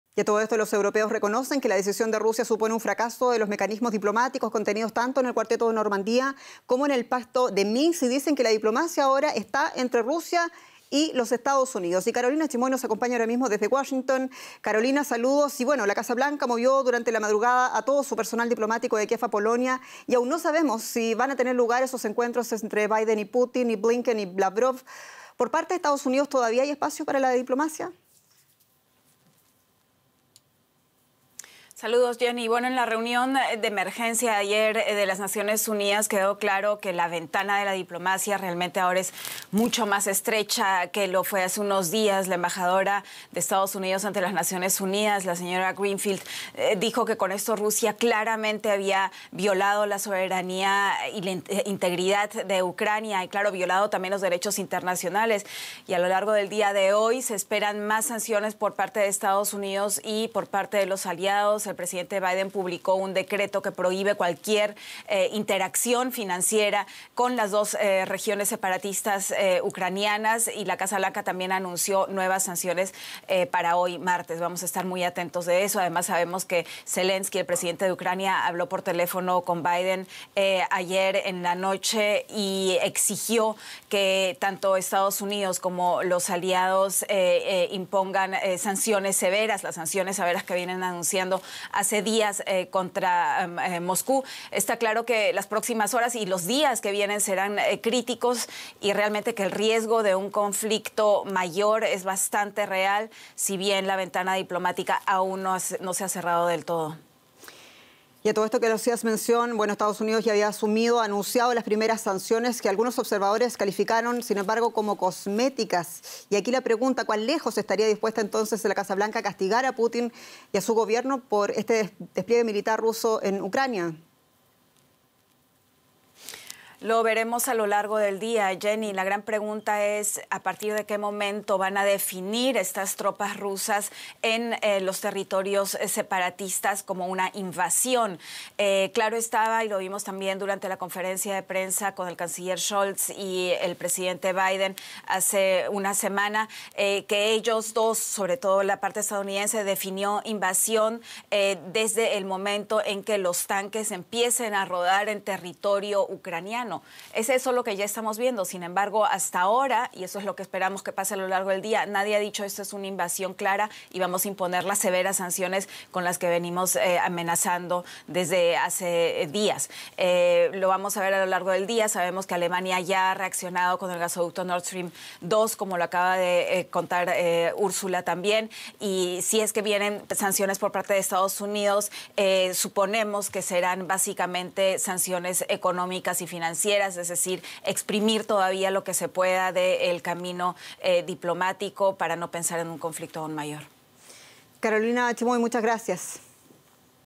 EE. UU. anunciará sus propias medidas contra Rusia
Para la Casa Blanca, Putin ha violado la soberanía e integridad de Ucrania, así como el derecho internacional. El personal diplomático de Estados Unidos ha dejado Ucrania. La ventana de la diplomacia entre ambos países es mucho más estrecha, analiza @CarolinaChimoy